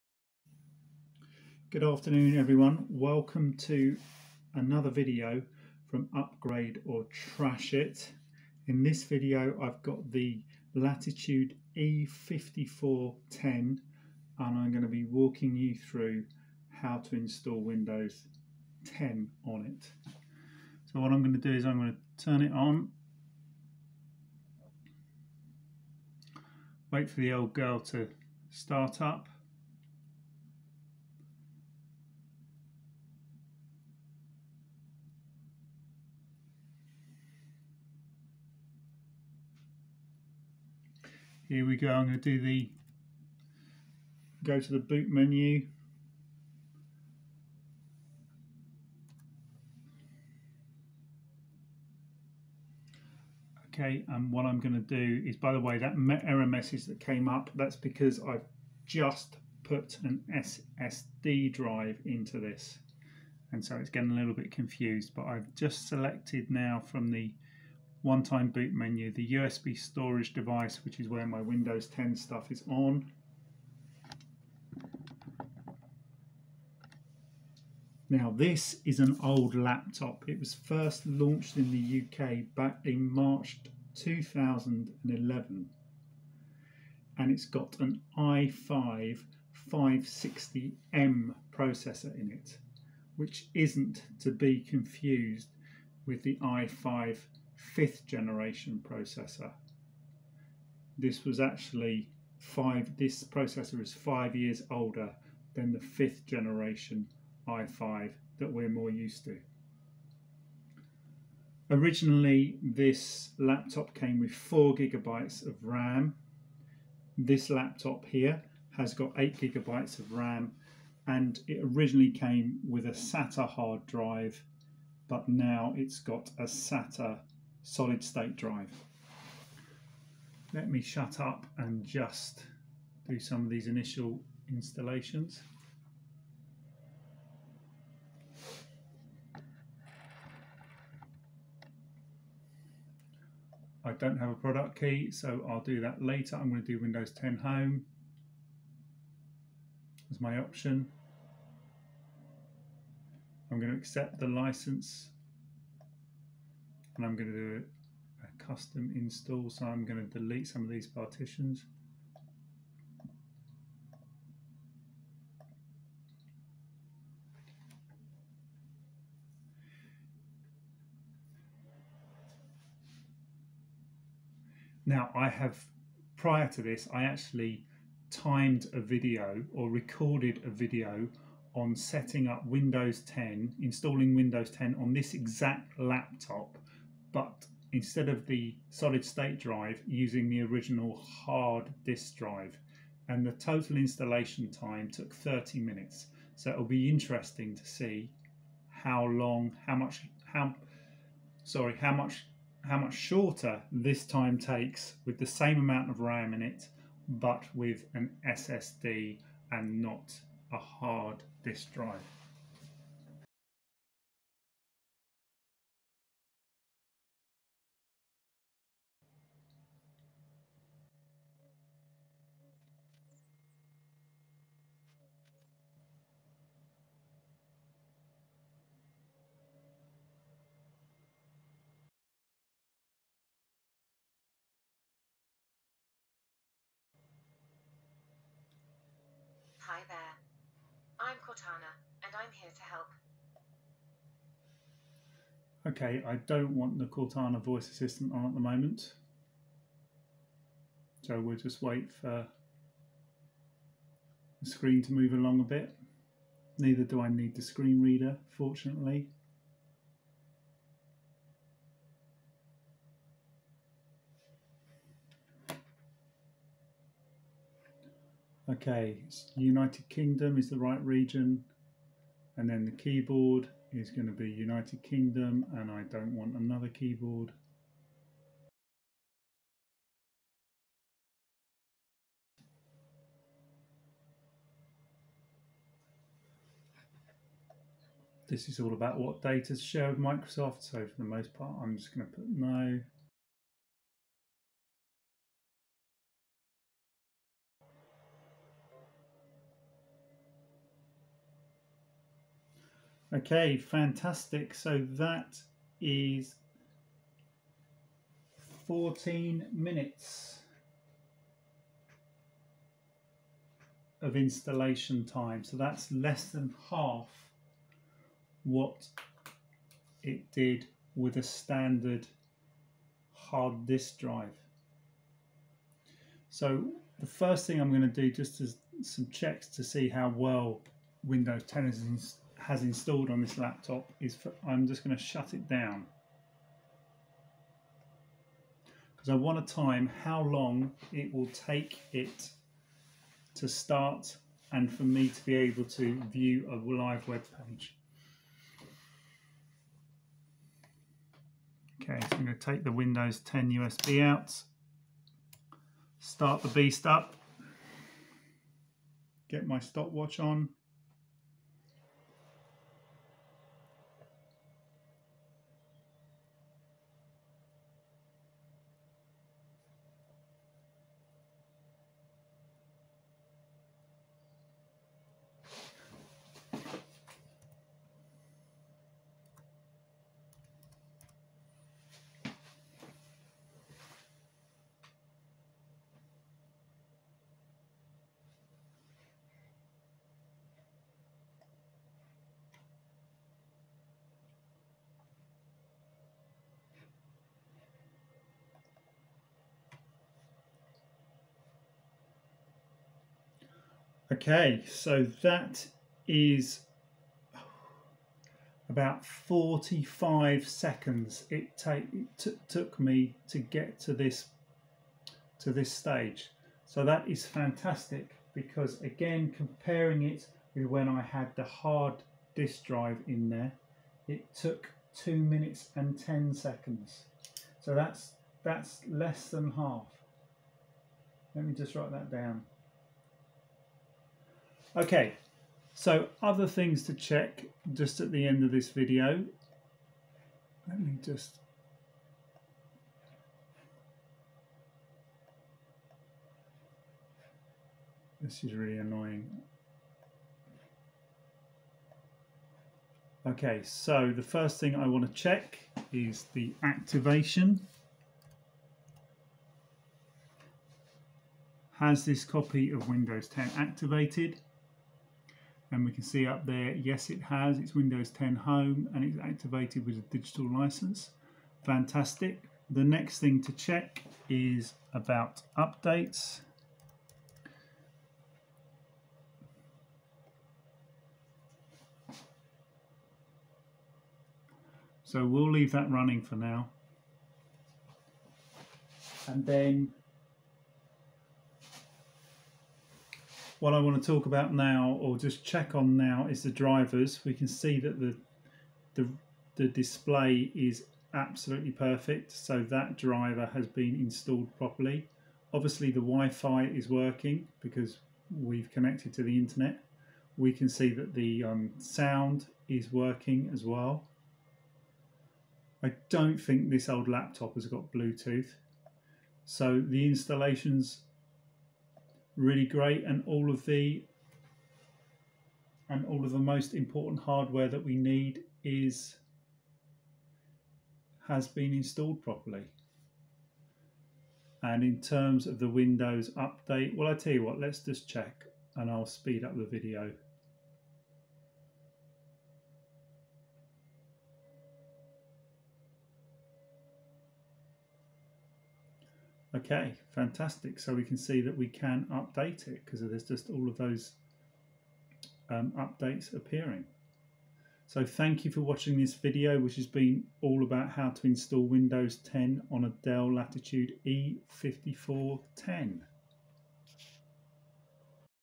Dell Latitude E5410: How To Install Windows 10 [With 8GB RAM & An SSD]
The E5410 was launched in the UK in early 2011. It is a dinosaur. It has an Intel i5 560M in it but you shouldn't confuse it with the i5 5th gen processor because it is so much older!

I have upgraded this laptop, to make it as fast as possible, so that it has 8 GB RAM in it and an SSD drive as opposed to a hard drive in it.

And it really helps! When I installed Win 10 on this laptop with just a hdd in it the installation took 30 minutes whereas with an SSD it took 14 minutes.

The SSD cut the start time down from 2 minutes 10 seconds to 45 seconds.